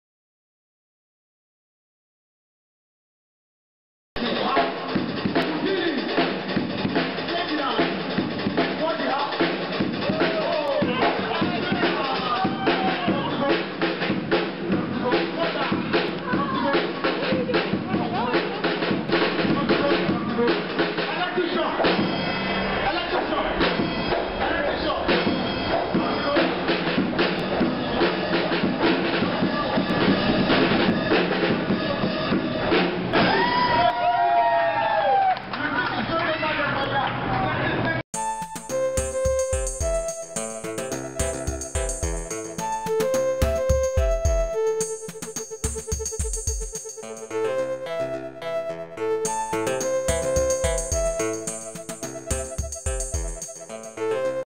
Subway Improvisation.wmv
Subway station improvisation entertainment,Drummer and HipHop dancer, at 125th Street Lex. Ave. station. This is the beginning of a series of subway performances.